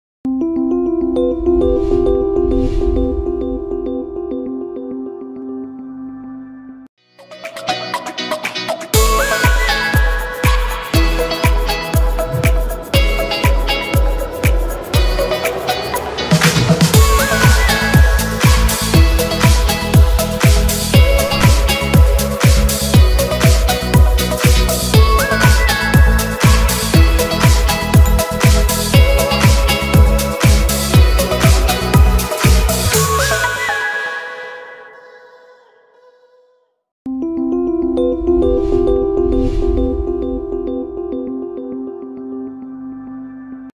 EBEL - the NEW Sport Classic in mint blue and praline l Jura Watches
Discover a stunning new addition to the Ebel Sport Classic collection. Available now: